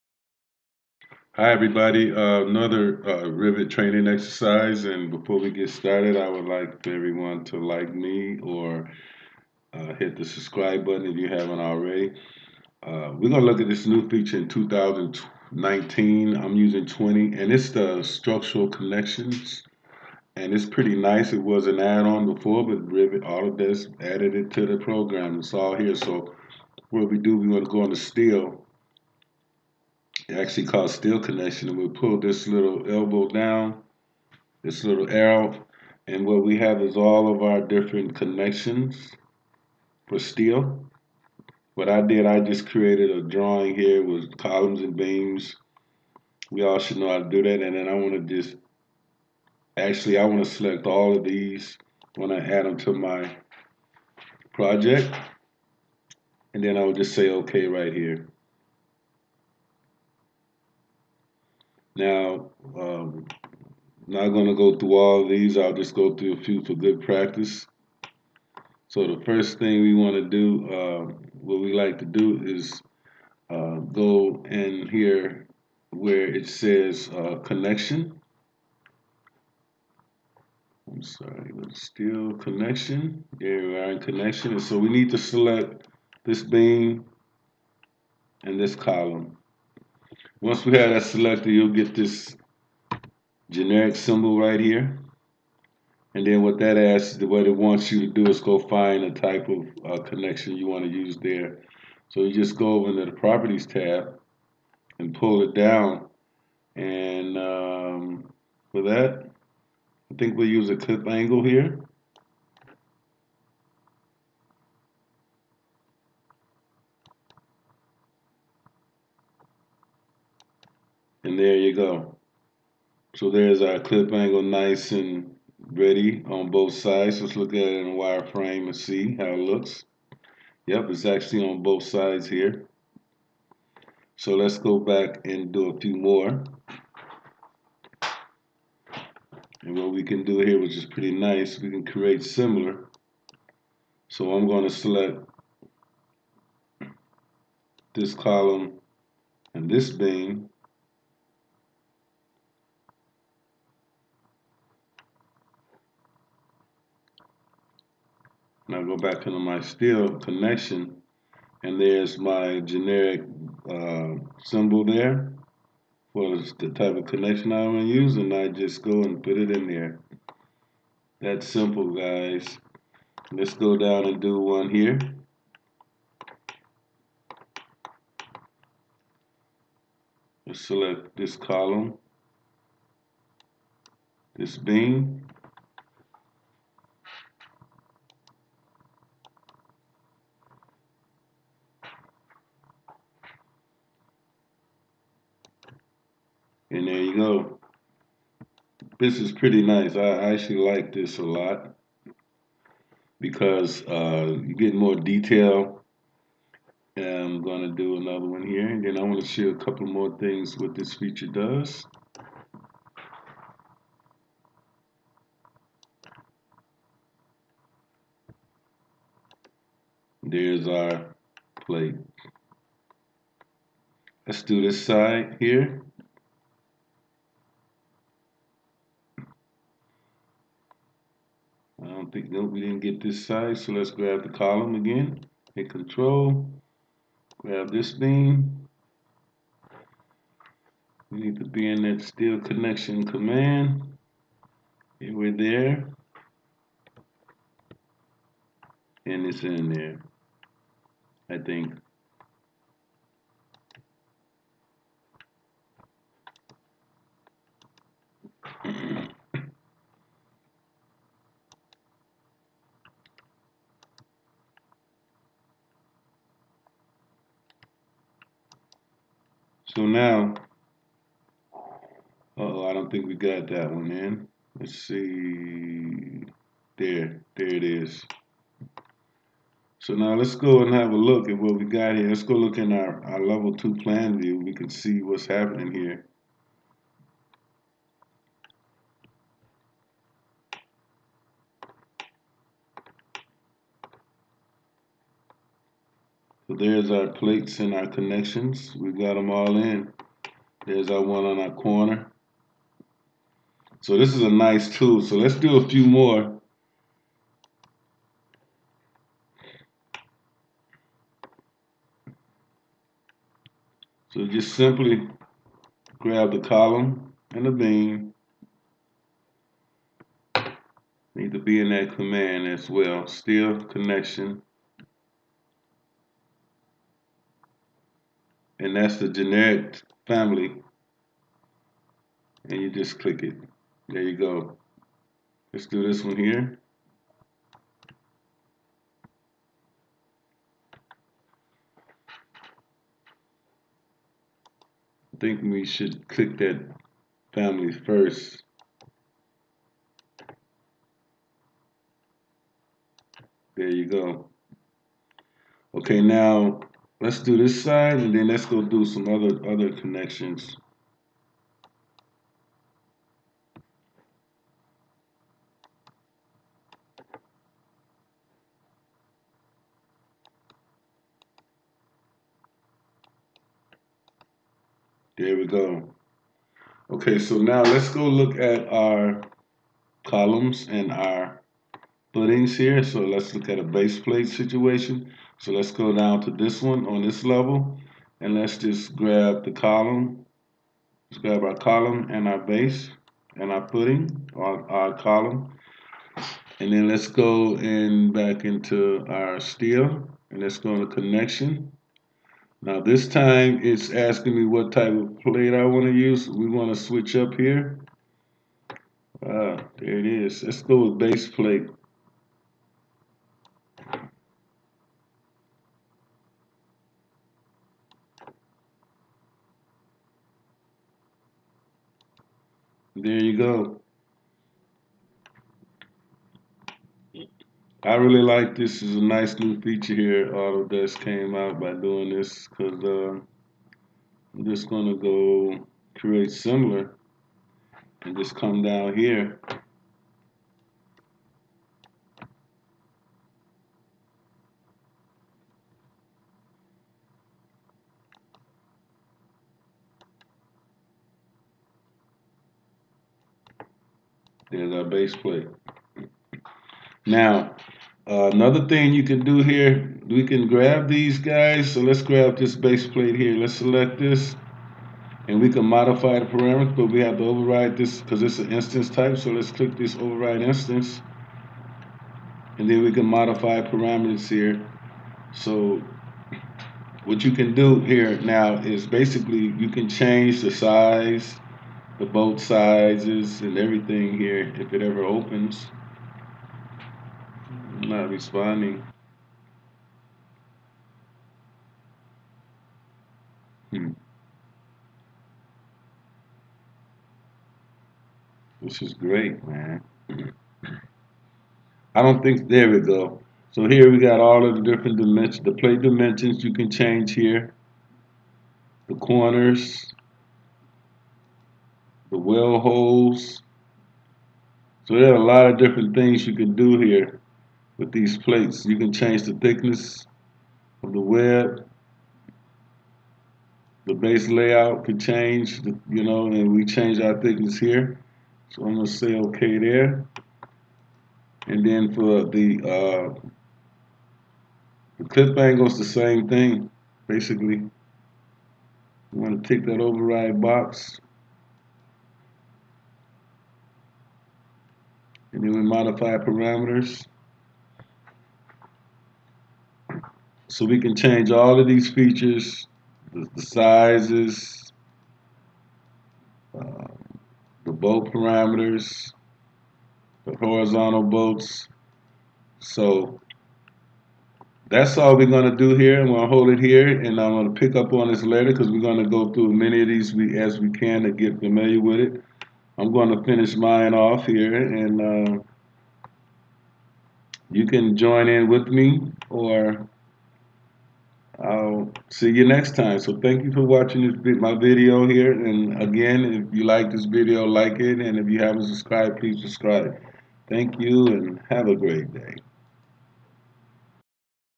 Revit Structure Steel Connections
Revit steel connections. How to use steel connections add-on.